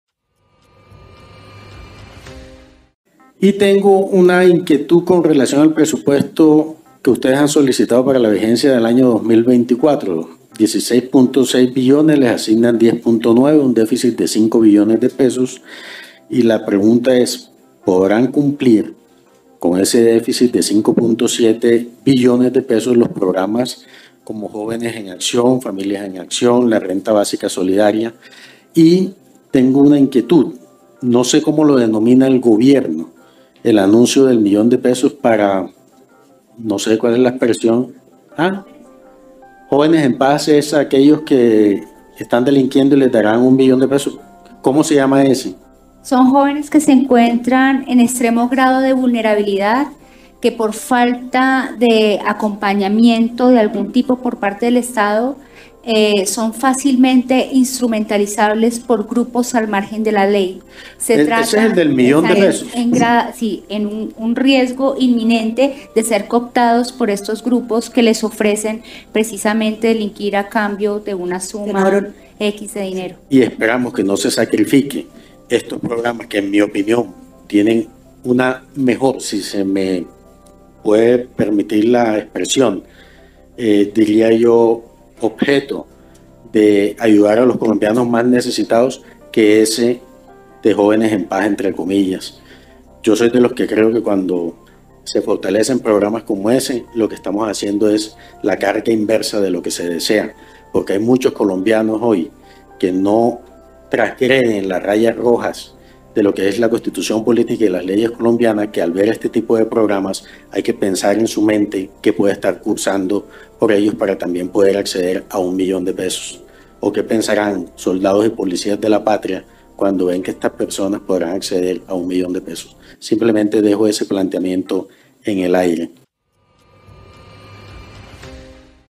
URGENTE PREGUNTA PARA DIRECTORA DEL DPS 📢 Honorio Henríquez
Pregunta para directora de DPS : ¿déficit de 5.7 billones para 2024, lograrán cumplirle programas sociales como Flias en Acción y Jóvenes en Acción? ¿O por el contrario priorizarán “jóvenes en paz”? Dónde ofrecen 1 millón de pesos a quienes delinquen. ¡Qué ironía!
HONORIO HENRÍQUEZ